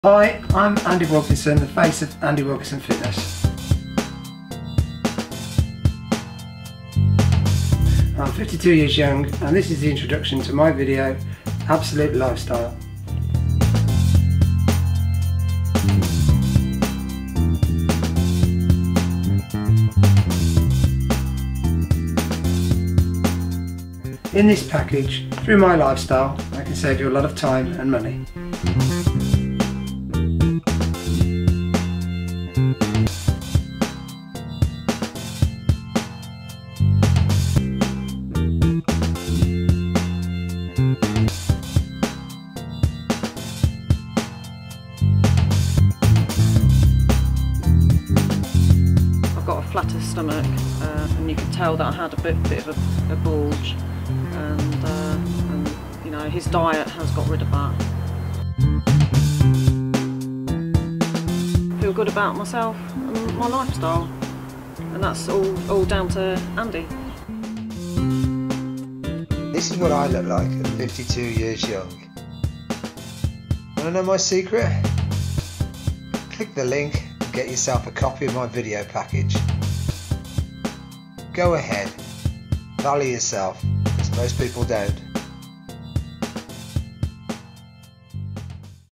Fit at 52 - The Secret to My Body (intro)
Hi my name is Andy Wilkinson and I have been in the fitness industry for over 30 years. This is the introduction to my video package, which teaches you the secret to having a body like mine at any age!